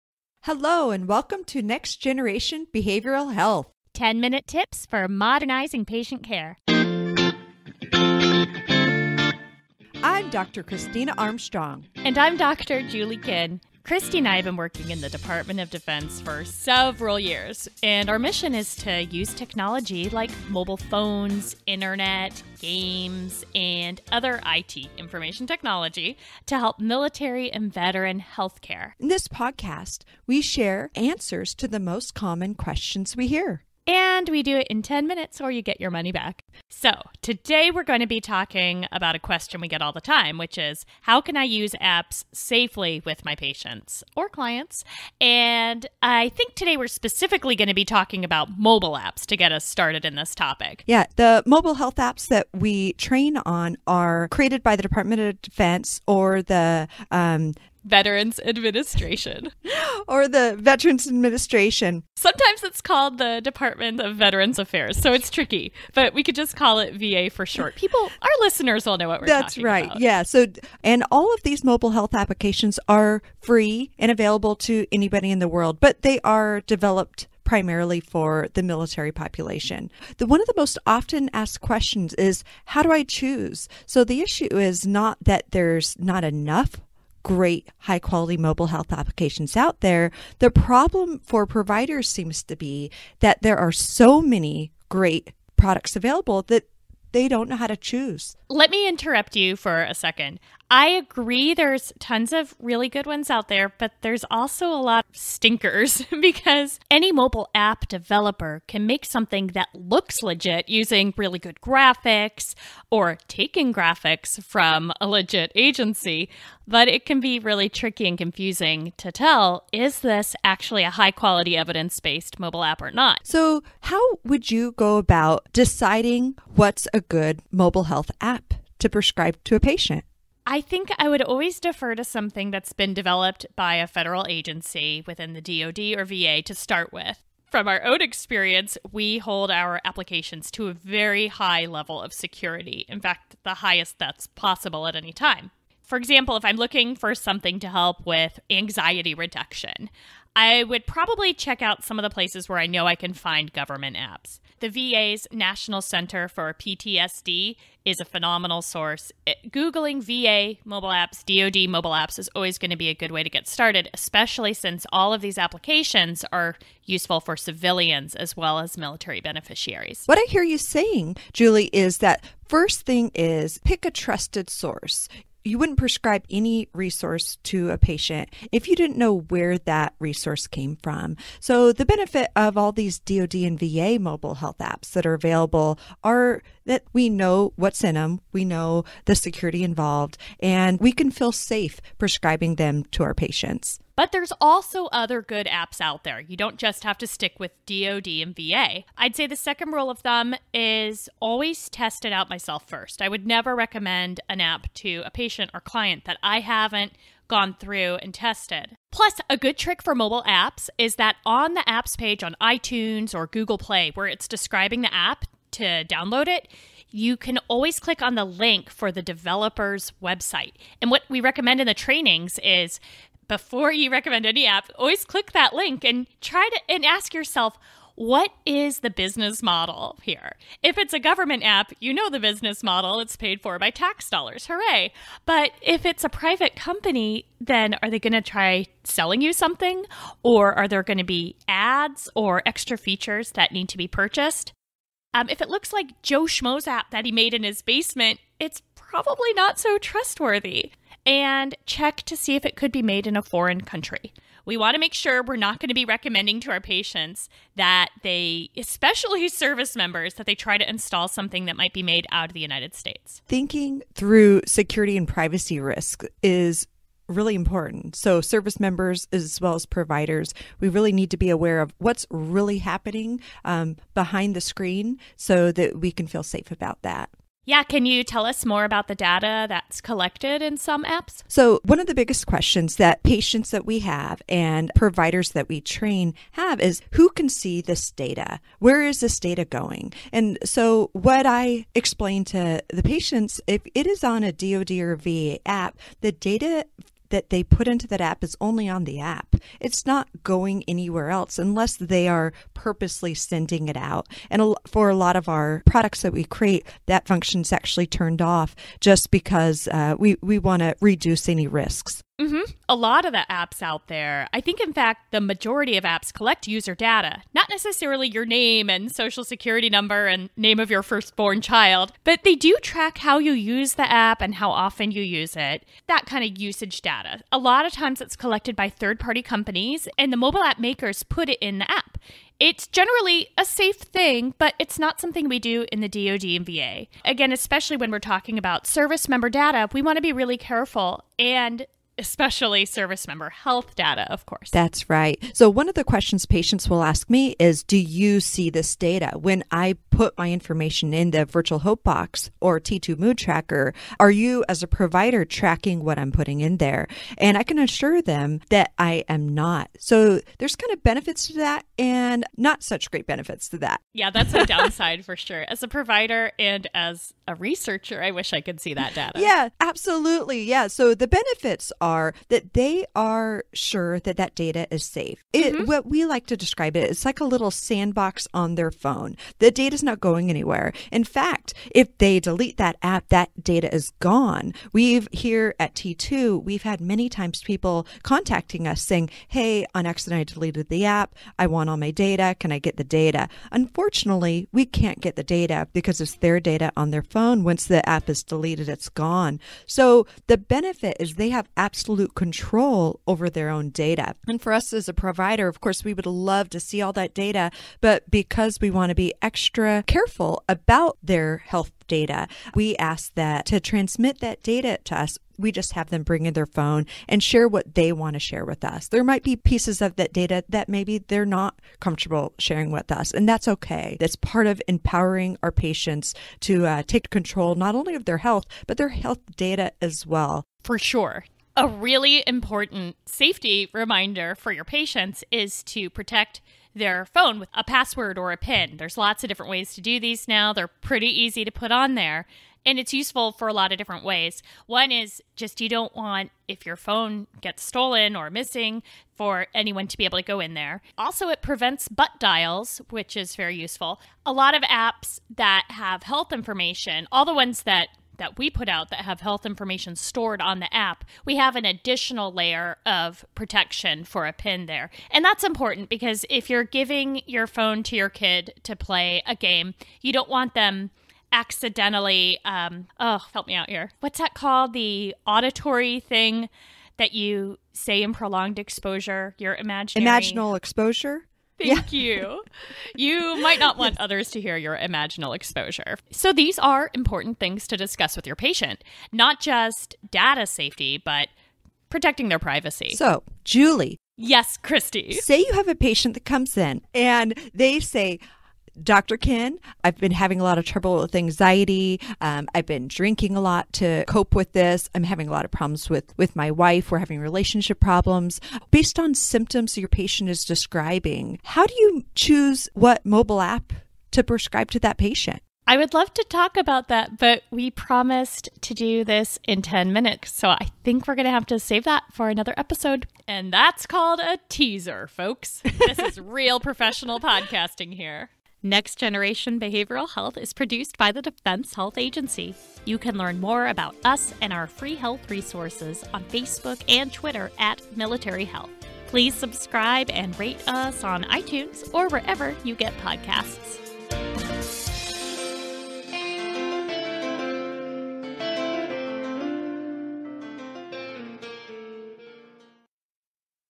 Mobile Apps Safety (Next Generation Behavioral Health Podcast)
Security and privacy issues in the clinical integration of mobile health are a top concern for providers and patients. Particularly in the military, providers and patients have a heightened awareness of security and privacy. In this episode, Dr. Christina Armstrong and Dr. Julie Kinn discuss some of the most commonly asked mobile health security and privacy questions and share tips on how to help protect patient data on a mobile device.

DHA Connected Health
Defense Health Agency

Next Generation Behavioral Health is produced by the Defense Health Agency